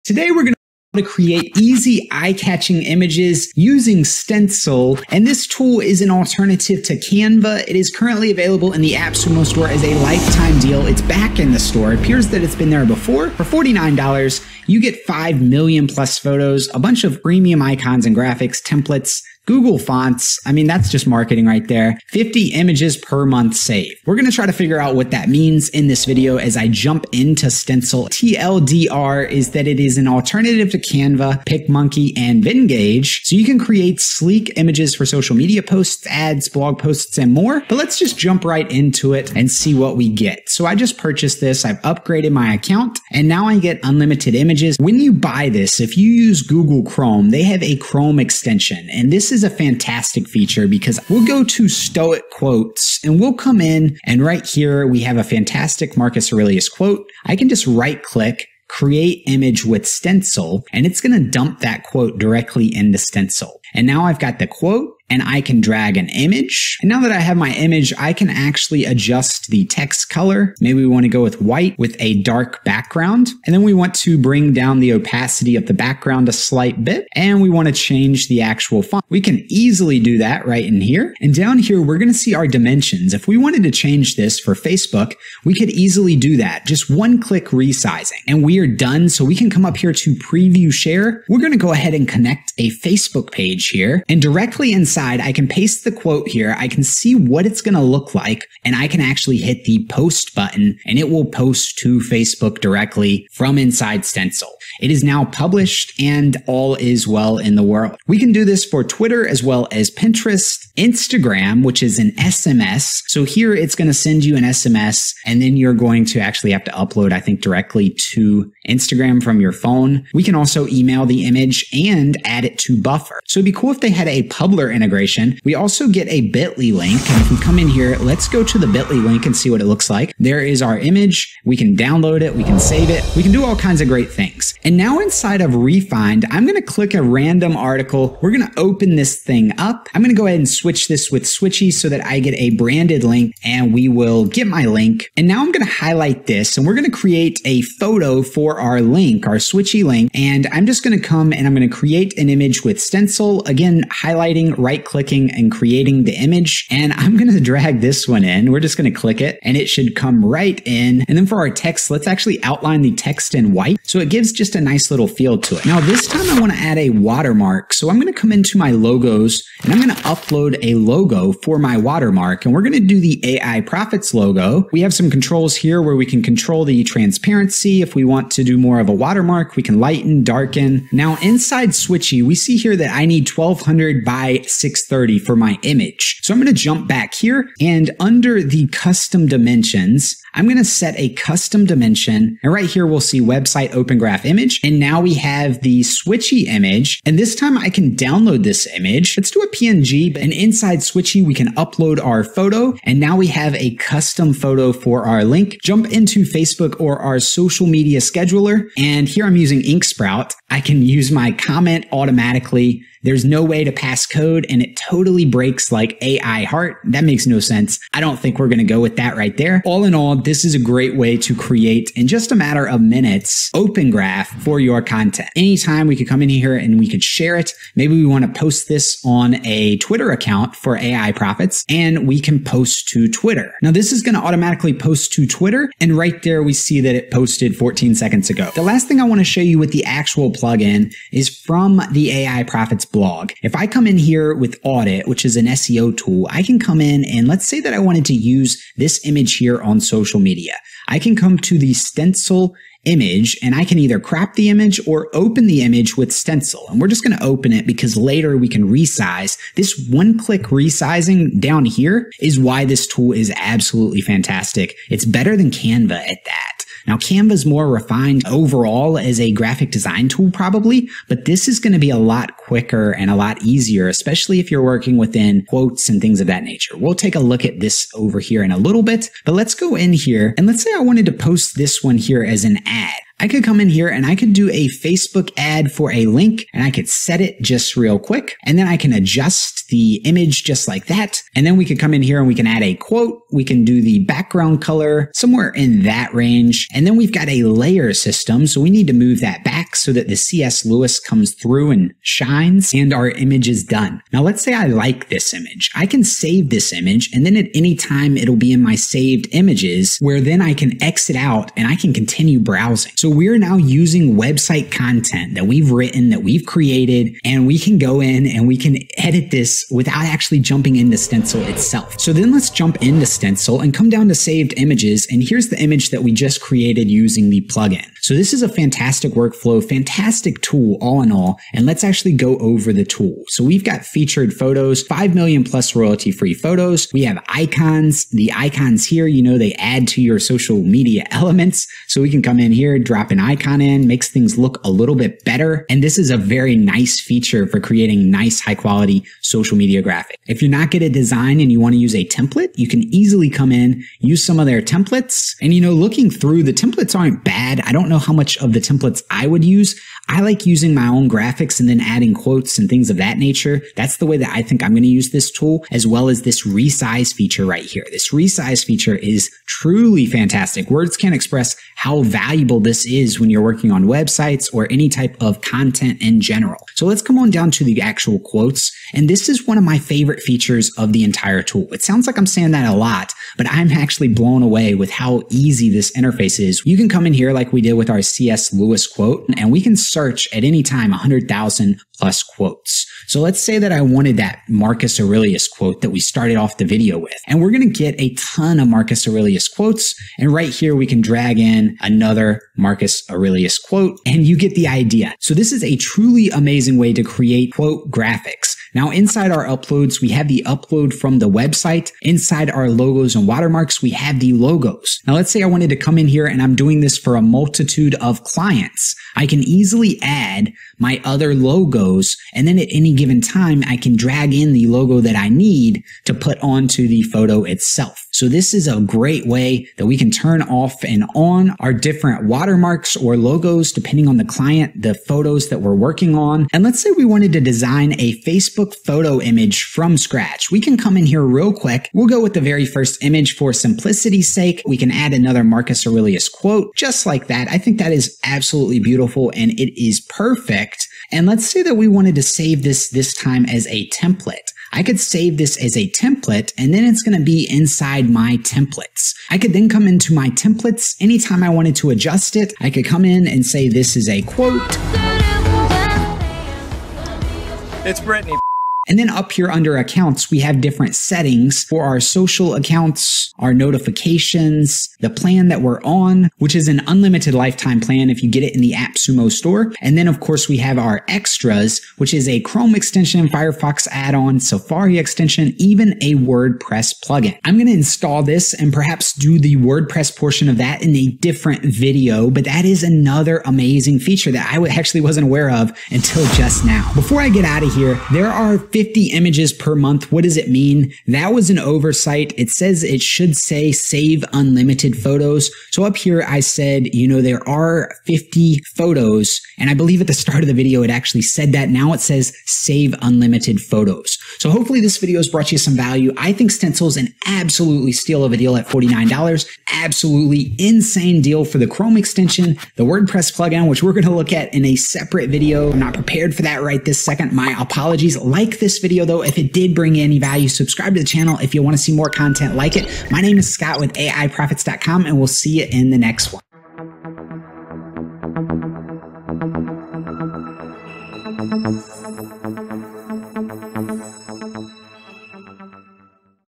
Social Media Graphics Made Easy With Stencil [Stencil Review / Appsumo Deal]
Now you can create stunning visual content at the press of a button.  No design skills needed.

In this video you'll discover a simple process that allows you to effortlessly create completely custom stunning social media graphics for any purpose.   I move fast through it, because if you've seen one graphic design tool, you've seen them all... but Stencil does some things the other tools miss - notably the easy to navigate chrome extension + the instant one-click-resizing.  These two features alone will make this WAY more valuable than the $49 price tag currently available in the appsumo store.

I hate making social media posts - it is why I often neglect my content on all the dozens of social media channels.  I find that to be a common theme...  I love canva and use it almost every day, but with Stencil, I think Canva may have a battle on its hands.  First, Canva feels far more mature.  Canva has the background removal features + the ability to share templates (sell) and far more integrations and workflows (publer).  It may not be an apples to apples comparison... But stencil has a surprisingly good Chrome extension + one-click-resizing and an easy to use social media push/publishing feature that I like the feel of better than canva.  It also has a quotes database + some fantastic stock photo choices and the ability to easily manage multiple brands and watermark the images accordingly.  Canva feels far more mature/complex but stencil looks like it is going to just WORK and make creating social media graphics easy again!

Social media graphics are an awesome way to make your blog more shareable, and Stencil is a tool that makes it easy for a small business owner to quickly create them.   Stencil is a free graphic design software that helps you create designs for Facebook, Twitter, Pinterest, and more. Stencil has over 5 million stock photos, 1.3 million icons and 1200+ templates so you can easily create beautiful, customized graphics for your business. Best of all, the software has a WP plug-in and a chrome extension - allowing you to truly save time and streamline your work.

Social media can be a great way to build brand awareness and customer loyalty, but it can also eat up a lot of your time. Stencil is a suite of easy-to-use tools to help you create social graphics, like banners, covers, and headers in minutes, without needing to use Adobe Photoshop or Illustrator. It's making graphic design fun again!